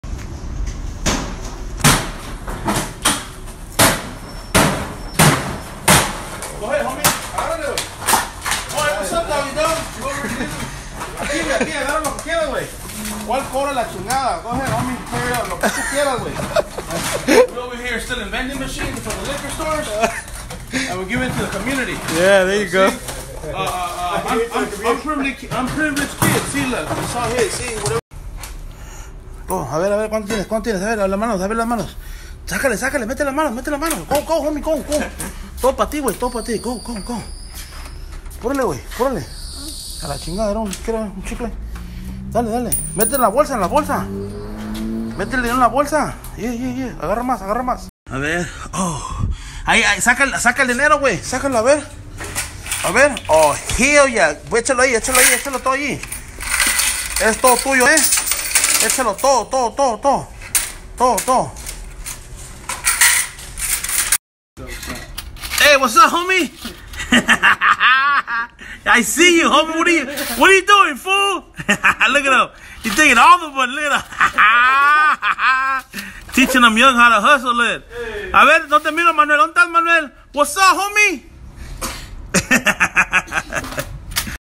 Breaking Vending machines
Took my kid Jose Manuel to open some vending machines that I found